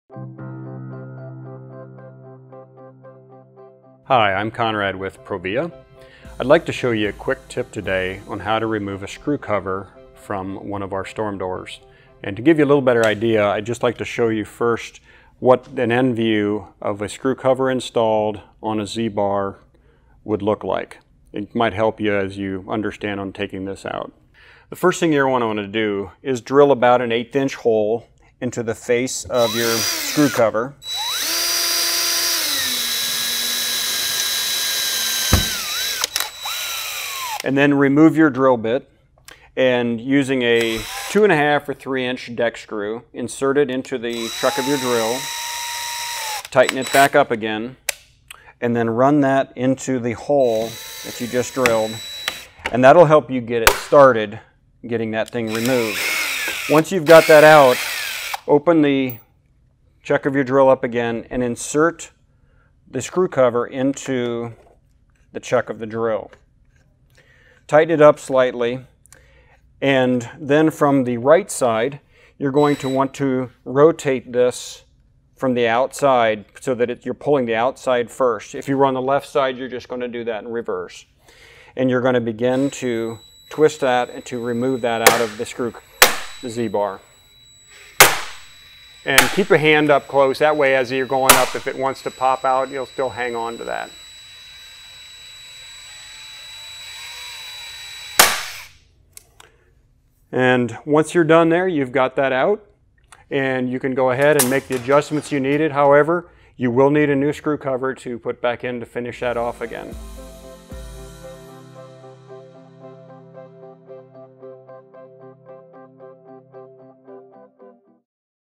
Storm Door Repair: How to Remove the Screw Cover
Using proper repair procedures can help lengthen the life of your aluminum storm or screen door.  ProVia Installation Support Manager Conrad Mast demonstrates a fast and effective way to remove the screw cover for aluminum storm and screen door repairs and maintenance. ProVia offers five lines of storm doors to meet every performance requirement and budget. Learn more about ProVia’s aluminum storm doors